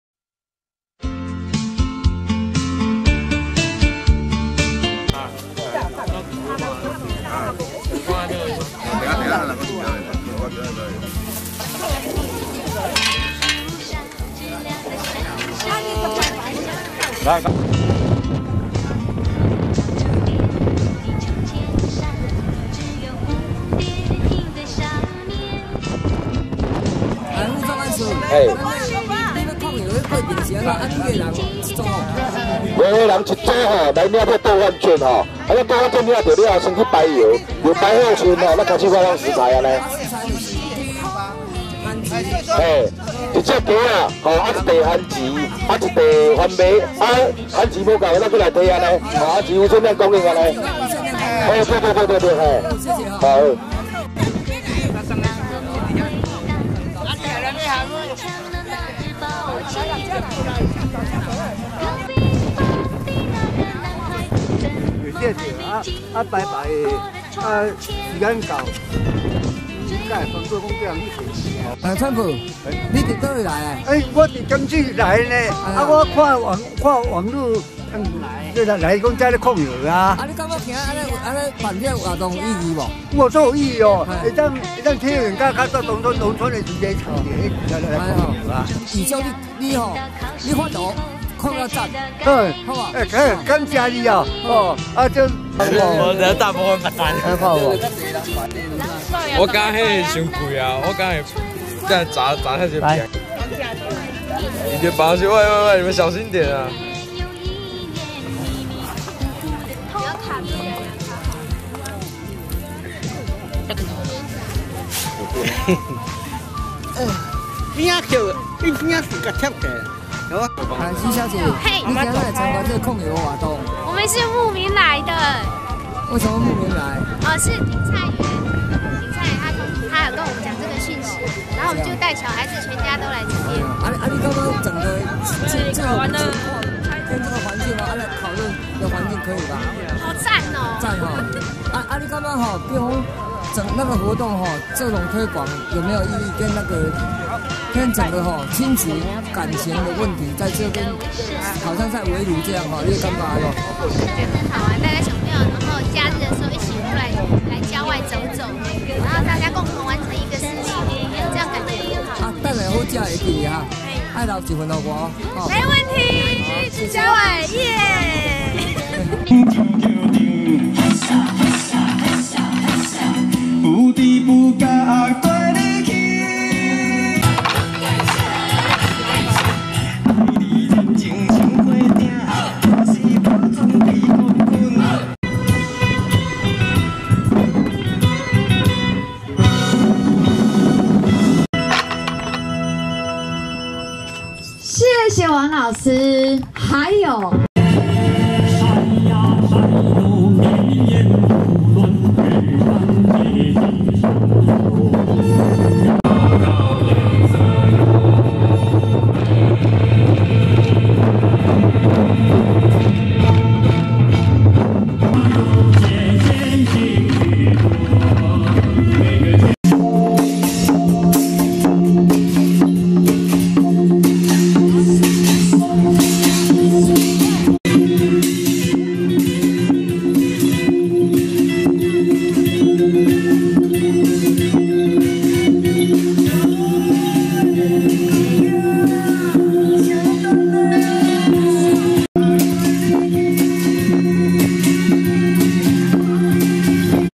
頂菜園-2009花現新港快樂希望-1
嘉義縣新港鄉，2009花現新港快樂希望，12月(19.20)日兩在板頭社區(19)、菜公村咬仔竹社區(20)日熱列展現，現場吸引成千上萬居民參與，熱鬧滾滾活動有表演節目、控窯、版畫、交趾燒彩繪等活動。